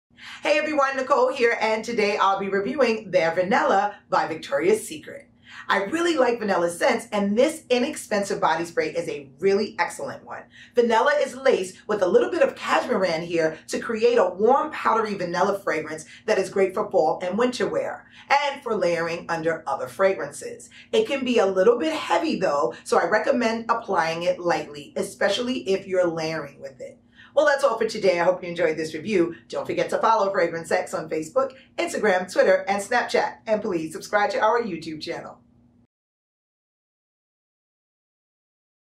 Victoria's Secret Bare Vanilla Perfume Review
Nicole reviews Victoria's Secret Bare Vanilla by Victoria's Secret.

TRANSCRIPT:
Hey everyone, Nicole here, and today I'll be reviewing Bare Vanilla by Victoria's Secret.  I really like vanilla scents, and this inexpensive body spray is a really excellent one.

Vanilla is laced with a little bit of cashmeran here to create a warm, powdery vanilla fragrance that is great for fall and winter wear, and for layering under other fragrances.  It can be a little bit heavy, though, so I'd recommend applying it lightly, especially if you're layering with it.